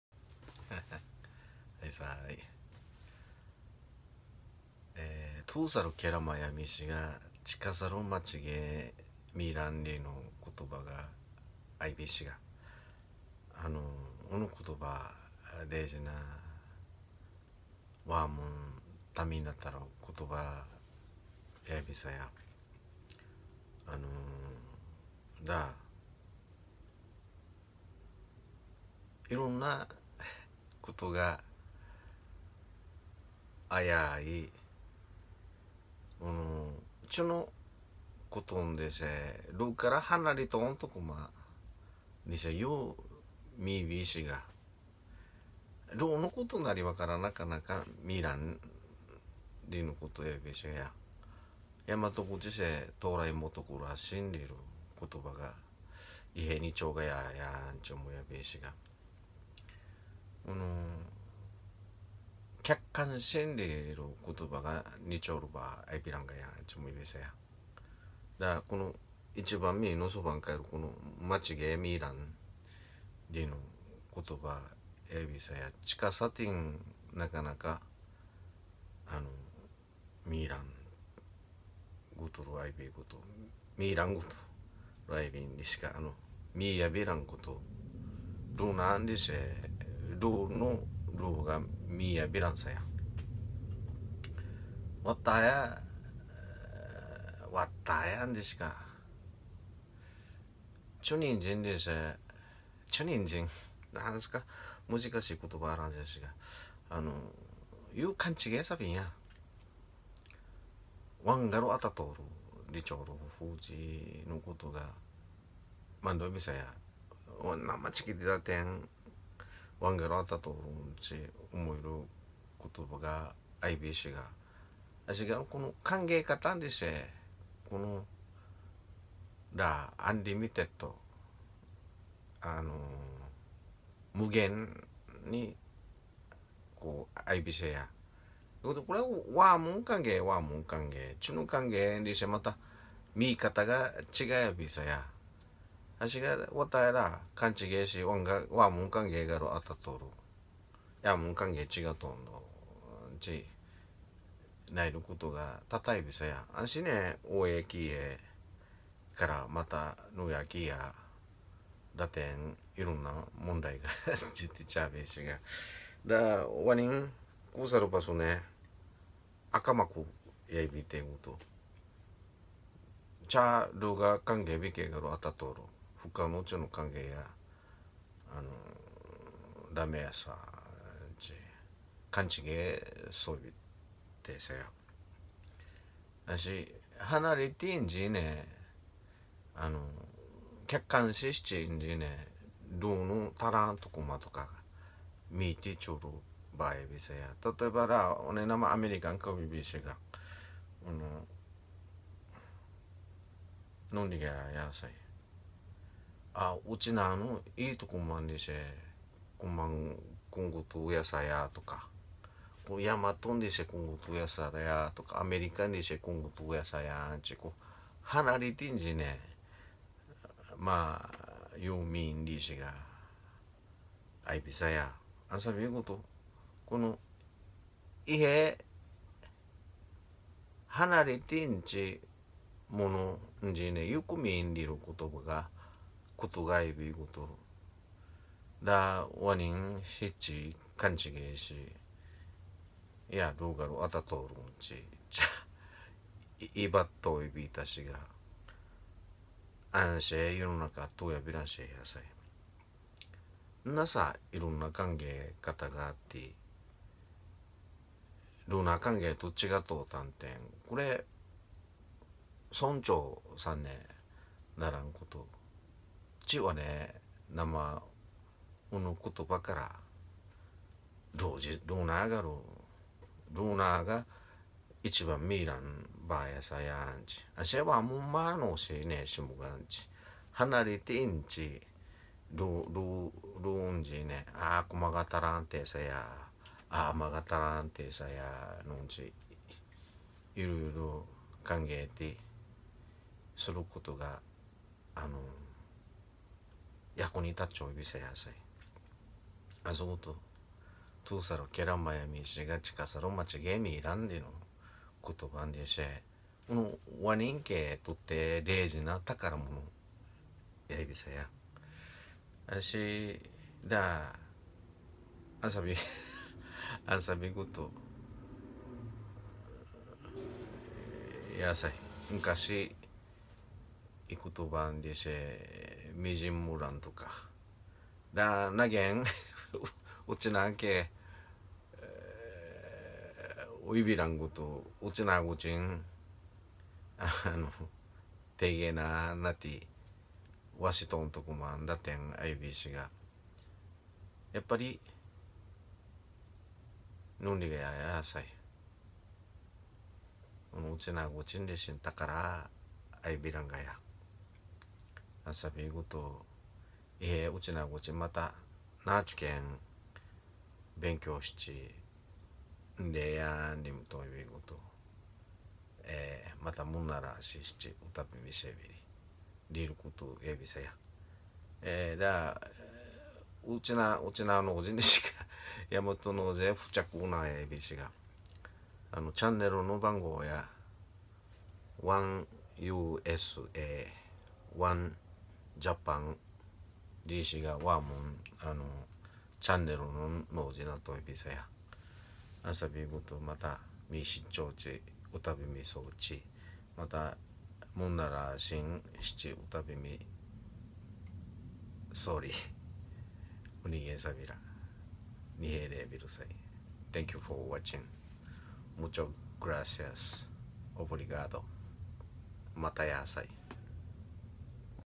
1usa1japan 沖縄の方言OkinawanLanguage
遠く離れた慶良間諸島は見えるが、近くのまつげは見えない#沖縄#方言#世界のウチナーンチュ